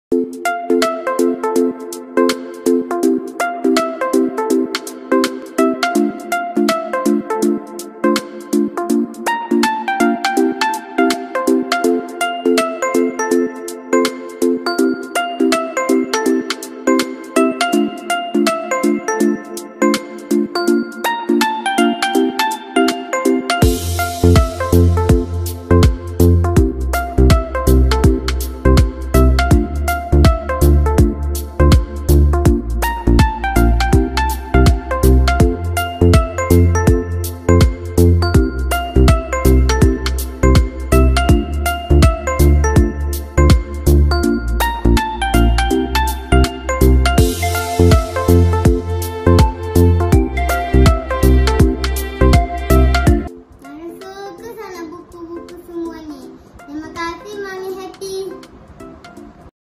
Testimoni Adik Naura Auni Buku Koleksi Teacher Ashikin
Waahhh! Adik Naura Auni pun seronok sangat menggunakan set 6 Siri Buku Cerita Puan Ashikin ni tau, siap ada video review lagi dari adik Naura! Terima kasih adik Naura! 🥰 @nauraboniey

Adik Naura kita dah tak buntu tau musim2 pkp ni nak kena buat apa, sebab banyak buku buku cerita yang best-best dan berfaedah yang boleh digunakan musim pkp ni

Jom dapatkann dari ejen-ejen Mommy Happy sekarang